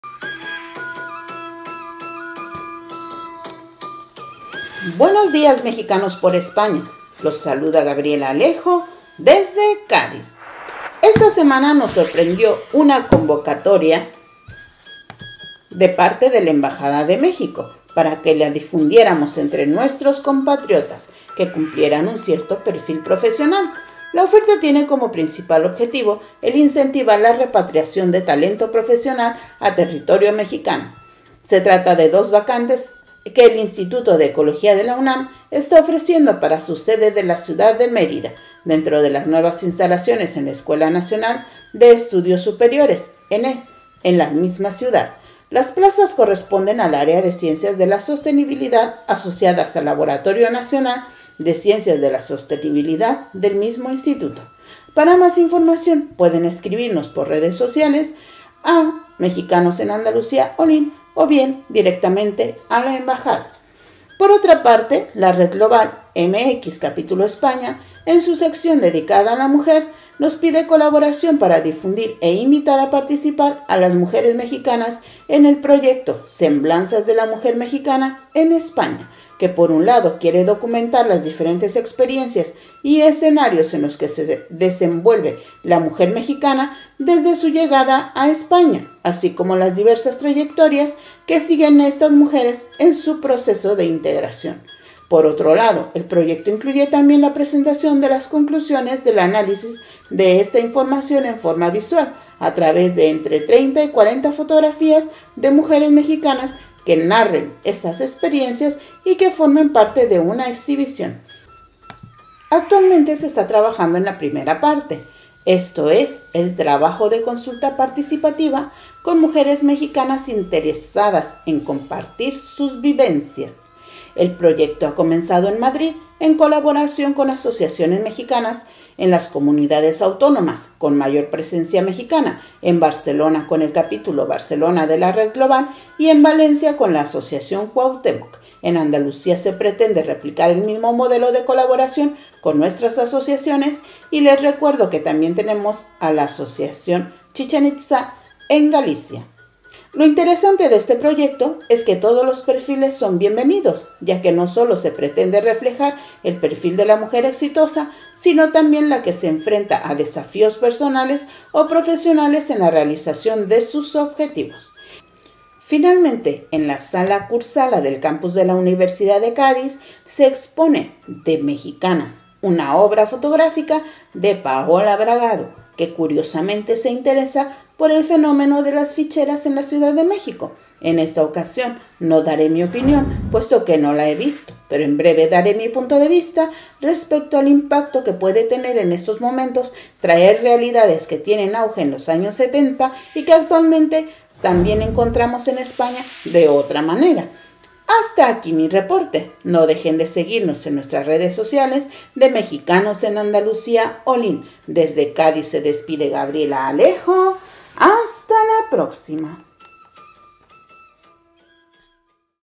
Semblanzas de la mujer mexicana 💢 Gabriela Alejo
💢 Gabriela Alejo informa:
💬 Semblanzas de la mujer mexicana

🎤 «Comunidades MEXicanas»
🎧 MxERADIO 02/05/2021

👁‍🗨 Presidente de la Asociación de Mexicanos en Andalucia "Olín", comparte sus actividades y las de otras asociaciones en España.